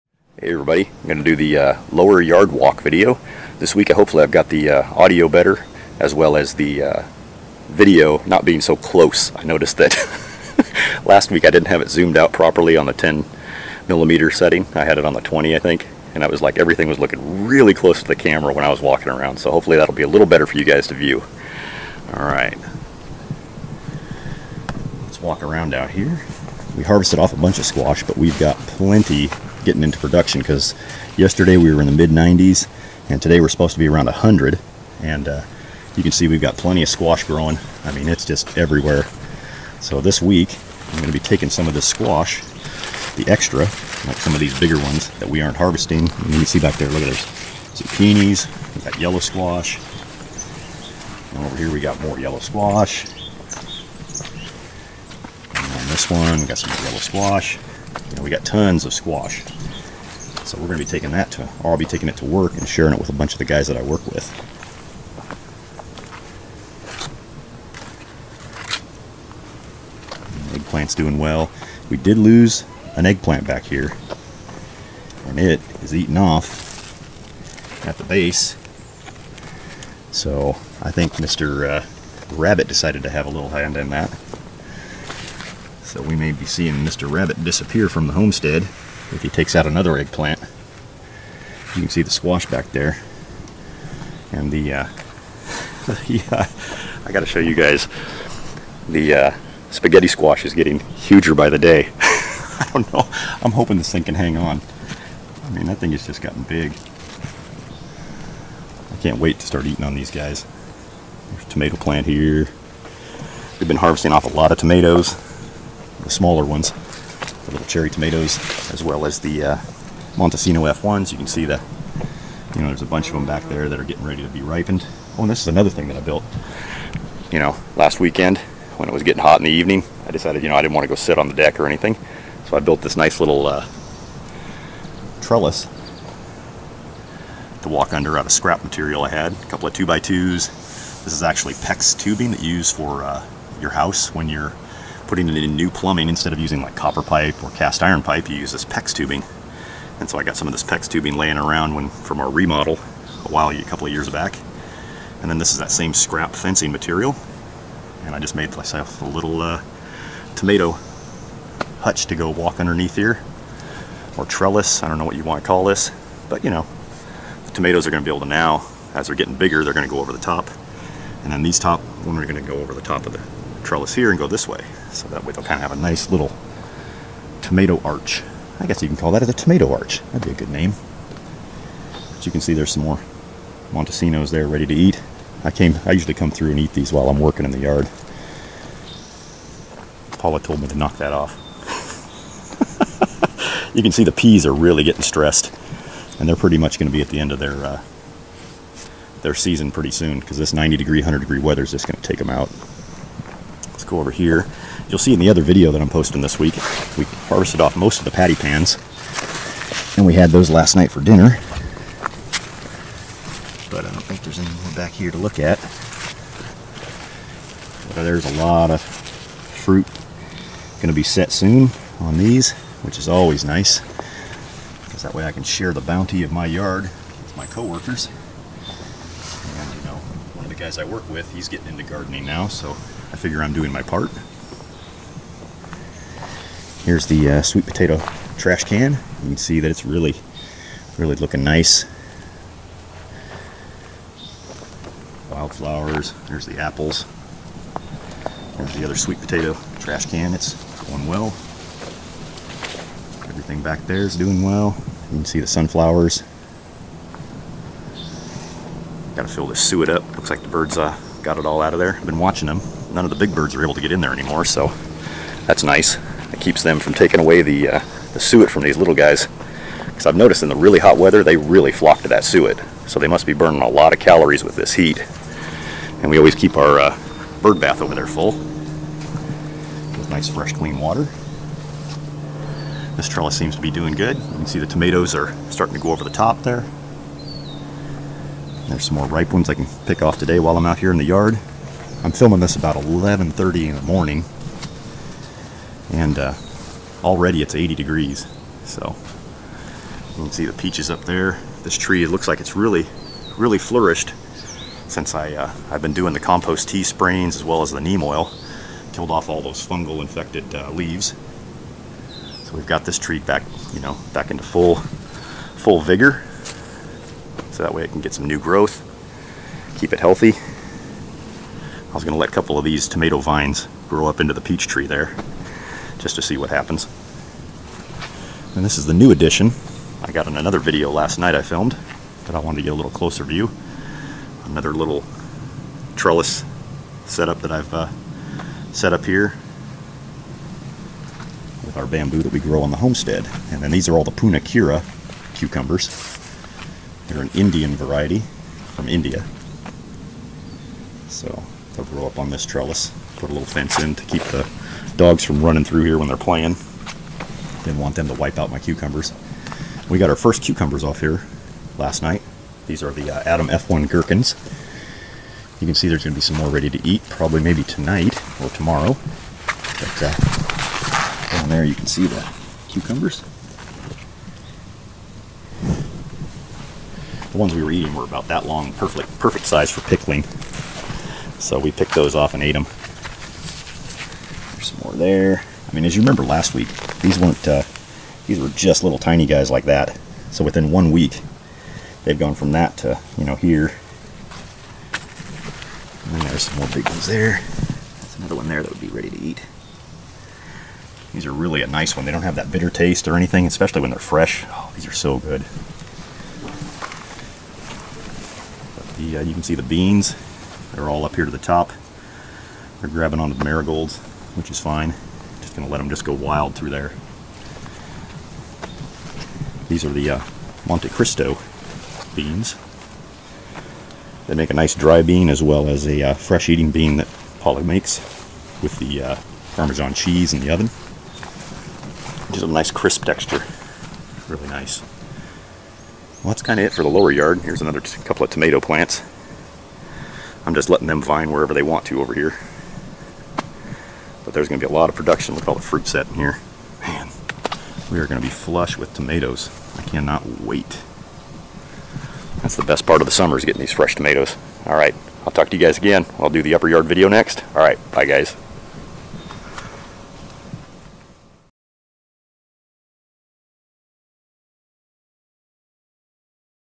Backyard homestead walk lower yard July 2018 week 27
Hi Everyone, things are really starting to take-off growing now that we're seeing consistent 90 temps during the day. (remember I'm still working on microphone setup this week, so might want to turn down the volume). Enjoy the video update! :-)